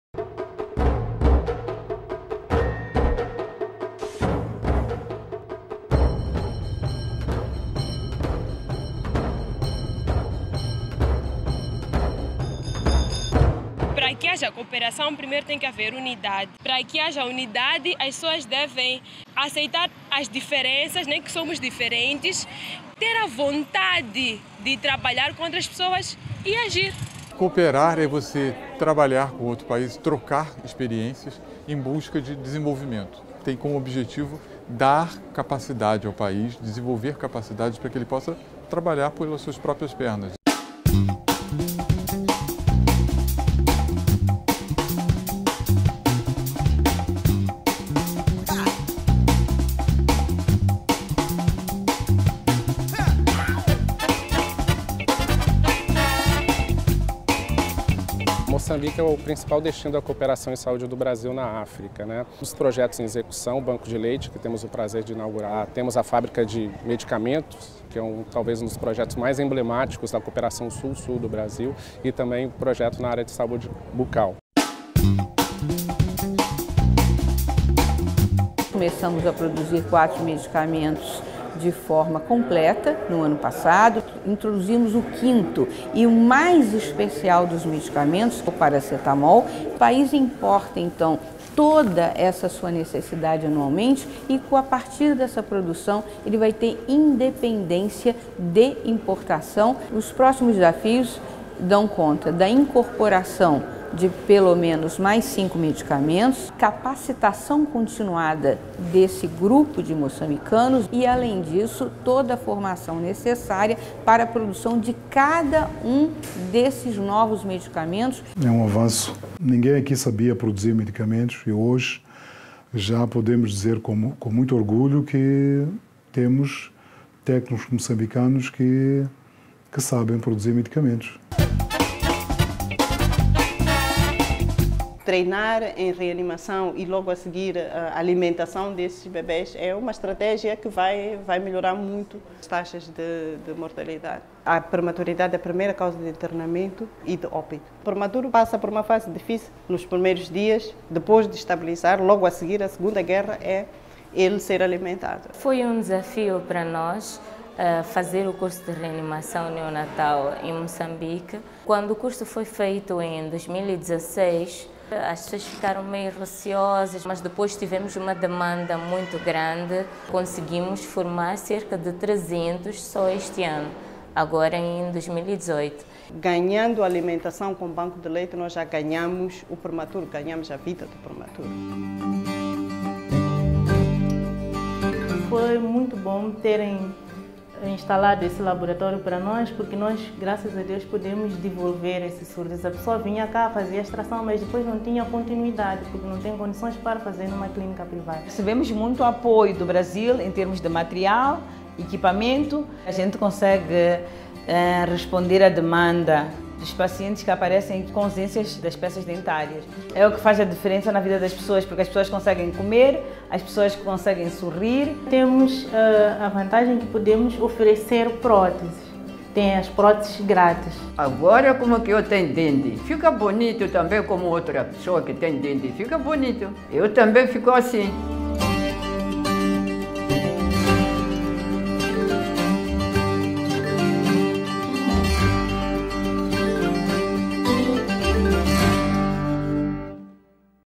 Conheça a cooperação técnica Brasil-Moçambique na área de saúde
A relação entre Brasil e Moçambique é marcada por fortes laços. O Brasil implementa projetos de cooperação técnica e humanitária no país africano em setores como: educação, saúde, agricultura e formação profissional.

Moçambique é um dos países com os quais o Brasil possui mais projetos de cooperação. Neste vídeo, produzido pela Secretaria Especial de Comunicação Social da Presidência da República (SECOM), é possível conhecer algumas iniciativas de sucesso na área de saúde implementadas pelo Brasil no país irmão.